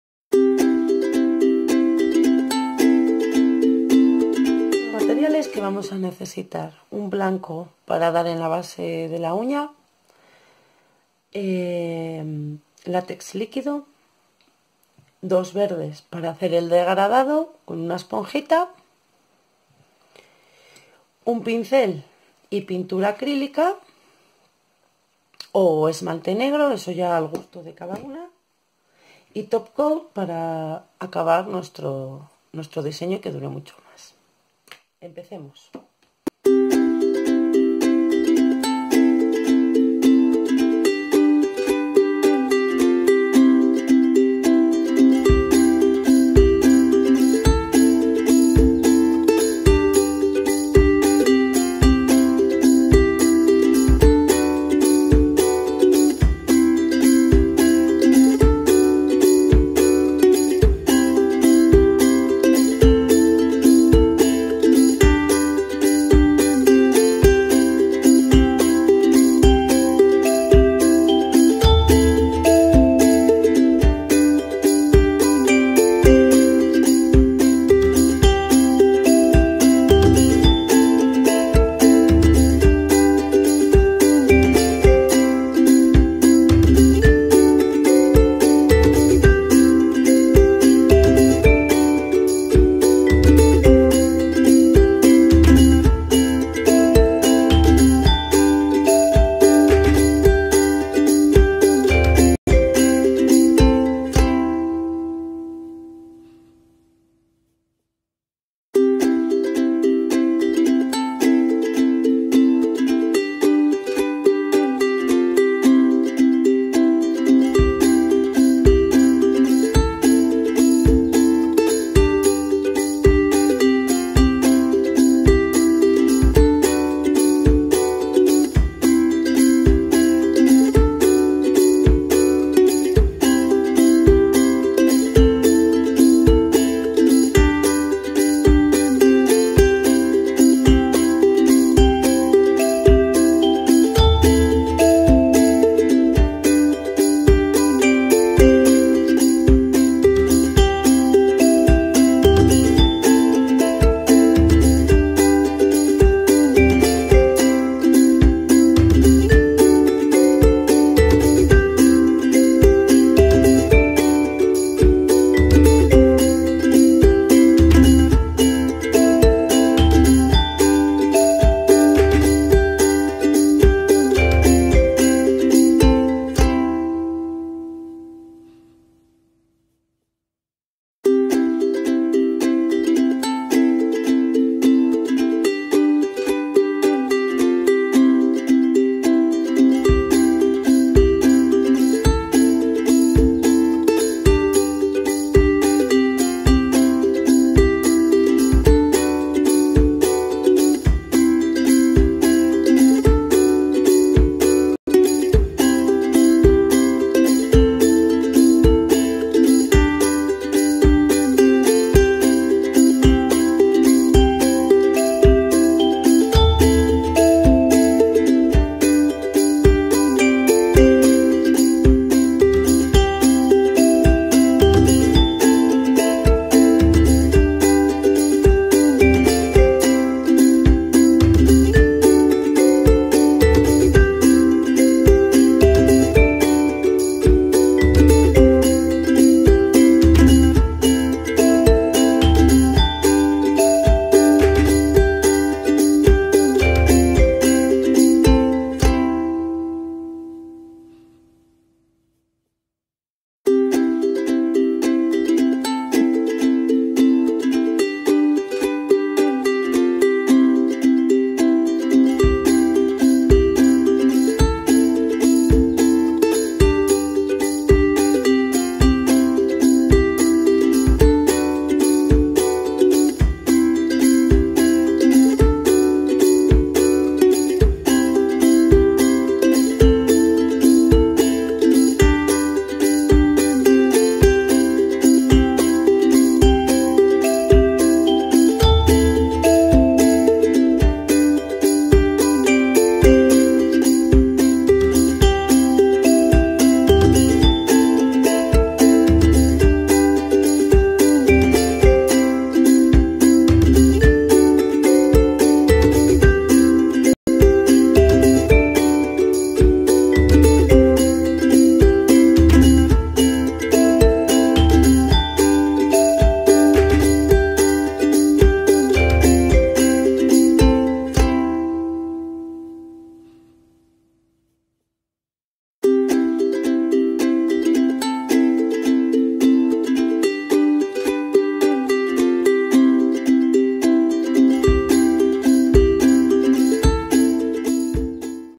::Nail Art mariposa monarca::
Aquí os dejo la manicura de esta semana para el reto de Instagram retocolorescosasdechicasvol5 el color que toca esta semana es el verde.

Si no queréis perderos ninguna manicura, os invito a que os paséis por mi Instagram